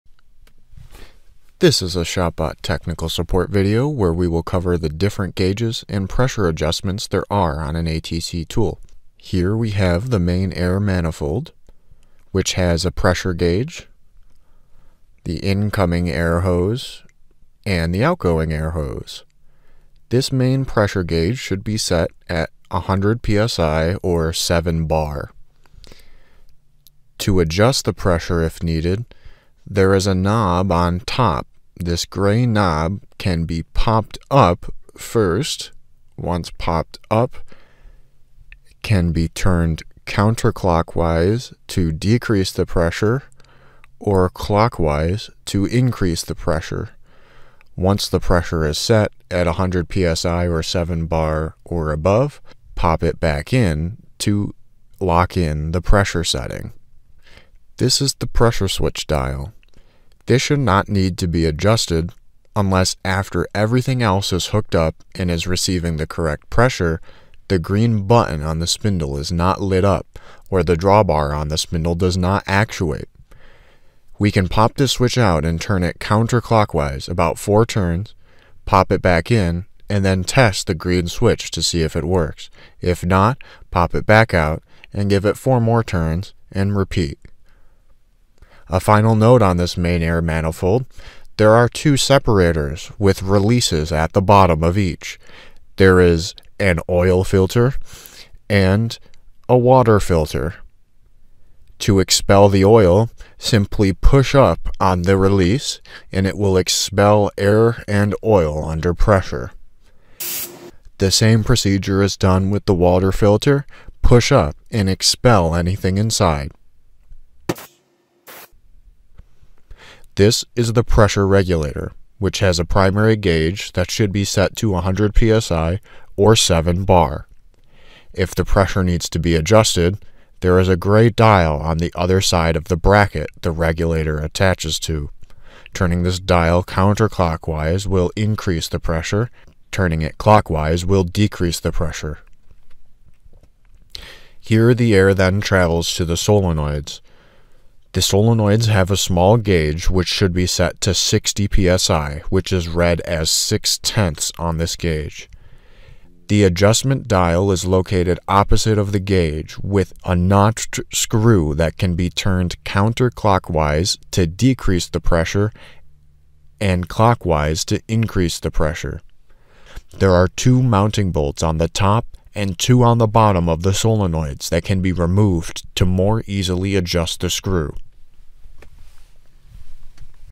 ATC - Gauge Set and Pressure Adjustment
This Technical Support video covers each of the pneumatic air components and and their gauges on an ATC tool - as well as the appropriate pressure settings and adjustment methods for these gauges.